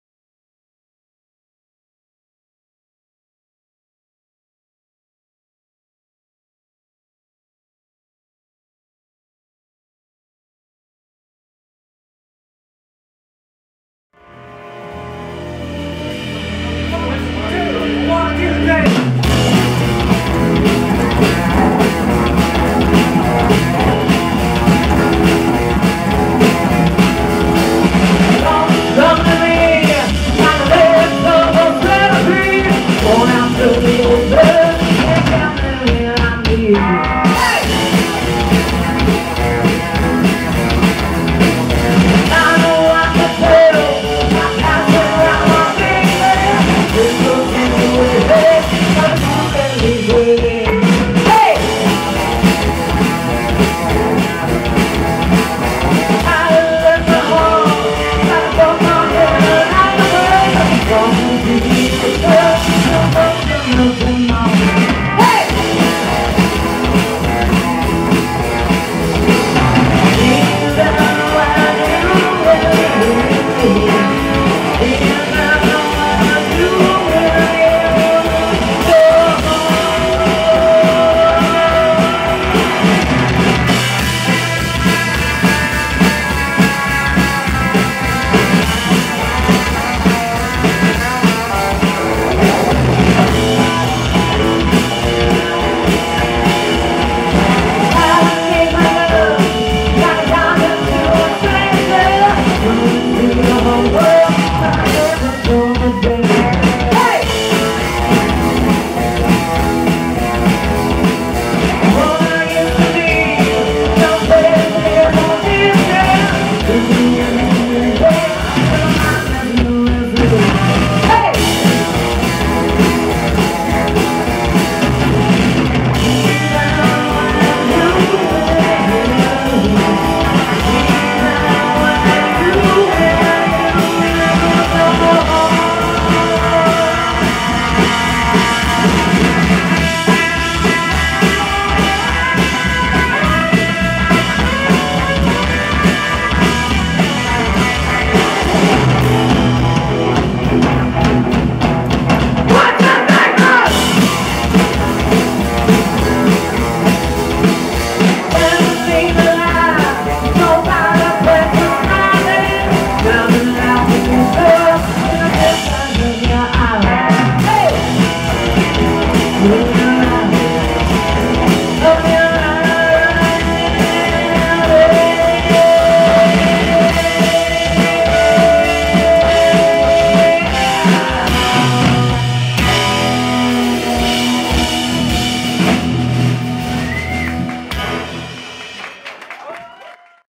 Screamin Rebel Angels Uk Tour playing at Trillians Newcastle.
Screamin Rebel Angels UK Tour. Playing @Trillians Newcastle.
12/9/2018.